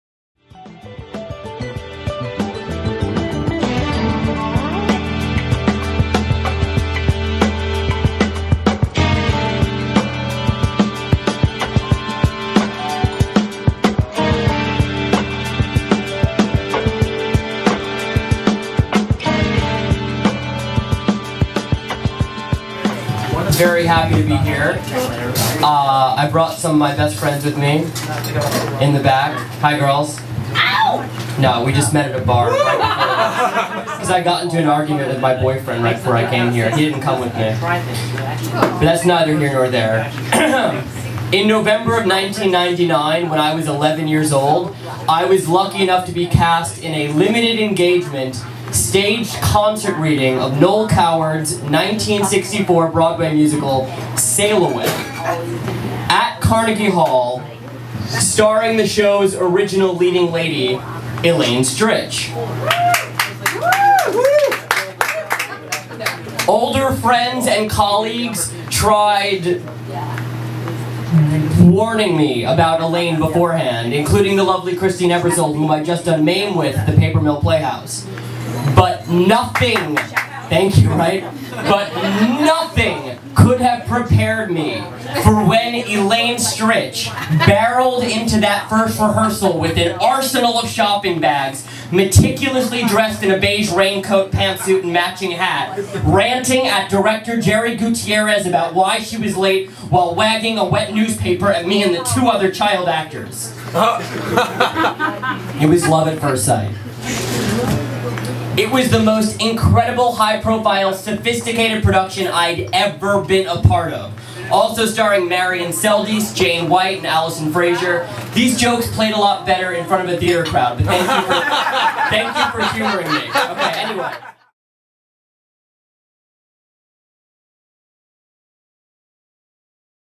Big City Stories: Paul Iacono talks about working with Elaine Stritch when he was a kid.
Actor Paul Iacono (Hard Times of RJ Berger, G.B.F) talks about working with the late Elaine Stritch when he was a kid. He learned from her that a true actor never lets anything--including broken bones--get in the way of a performance.

Filmed live at Big City Stories in NYC on 9/30/14. Big City Stories is a monthly storytelling show presented by Urban Cricket and hosted by Tommy O'Malley.